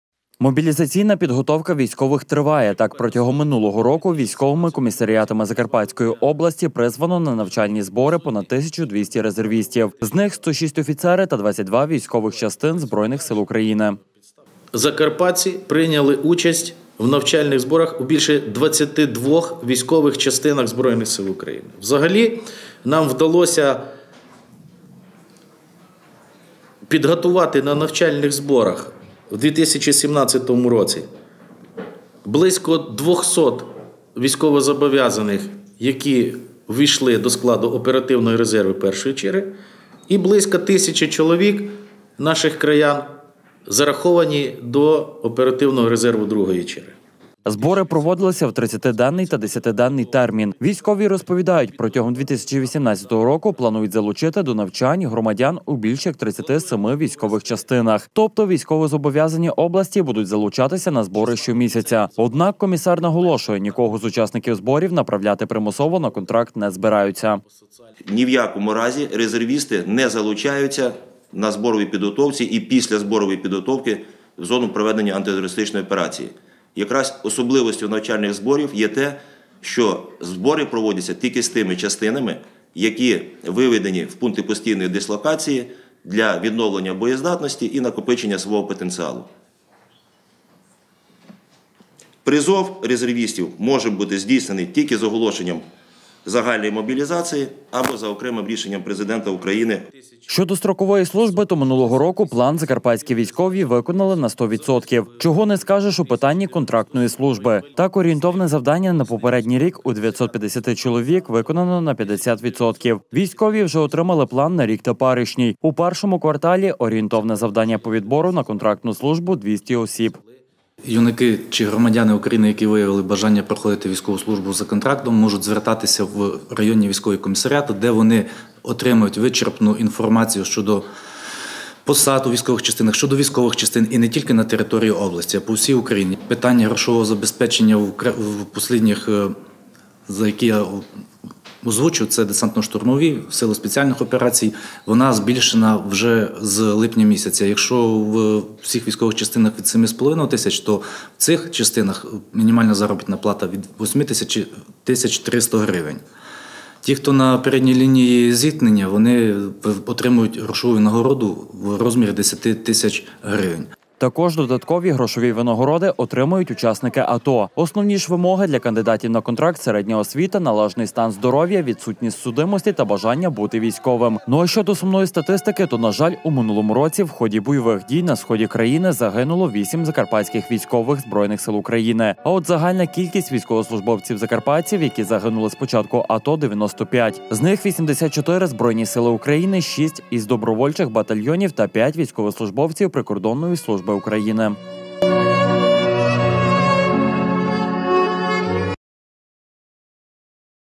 На Закарпатті підбили підсумки діяльності військових комісаріатів
Мобілізаційна підготовка військових триває. Так протягом 2017 року військовими комісаріатами Закарпатської області призвано на навчальні збори понад 1200 резервістів з них 106 – офіцери та 22 військових частин ЗСУ. Збори проводилися у 30-ти денний та 10-ти денний термін. Військові розповідають протягом 2018 року планують залучити до навчань громадян у більш, як 37 військових частинах. Тобто військовозобов'язані області будуть залучатися на збори щомісяця. Однак комісар наголошує, нікого з учасників зборів направляти примусово на контракт не збираються. Щодо строкової служби: то минулого року план закарпатські військові виконали на 100%. Чого не скажеш у питанні контрактної служби. Так орієнтовне завдання на 2017 рік, у 950 чоловік - виконано на 50%. Військові вже отримали план на рік теперішній , у першорму кварталі орієнтовне завдання по відбору на контрактну службу – 200 осіб.
Також додаткові грошові винагороди отримують учасники АТО. Основні ж вимоги для кандидатів на контракт – середня освіта, належний стан здоров'я, відсутність судимості та бажання бути військовим. Ну ,а щодо сумної статистики, то на жаль у минулому році в ході бойових дій на сході загинуло 8 Закарпатських військових Збройних сил України. А от загальна кількість військовослужбовців- закарпатців, які загинули з початку АТО – 95 . З них : 84- ЗСУ , 6 зі добровольчих батальйонів , та 5 військовослужбовців прикордонної служби України.